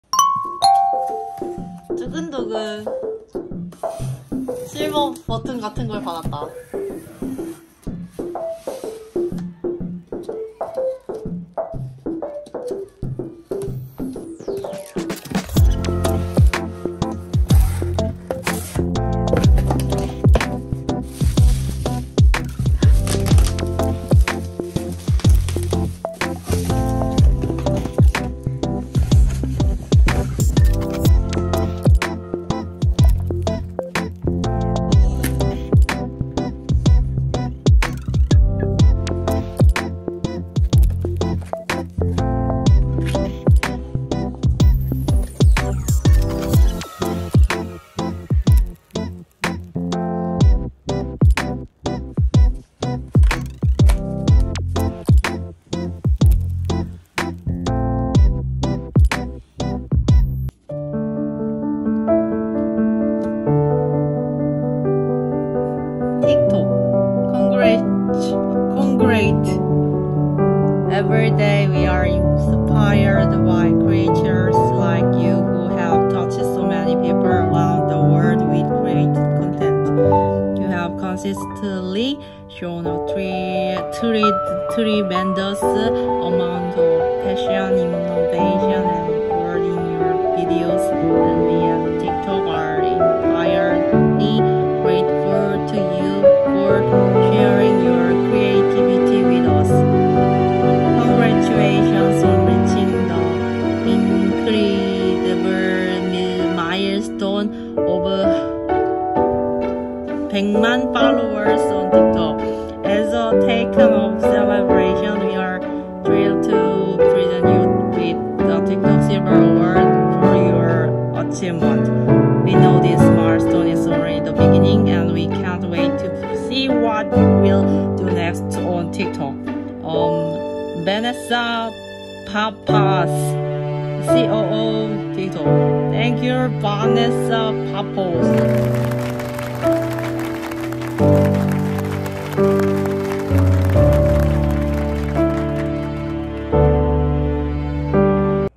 💥틱톡에도 실버버튼이 있다는 사실! 200만 틱톡커의 틱톡 어워즈 언박싱~!
❤ 틱톡에도 실버버튼이 있다는 사실! 아셨나요?
❤ 틱톡 실버 어워즈 언박싱~! :D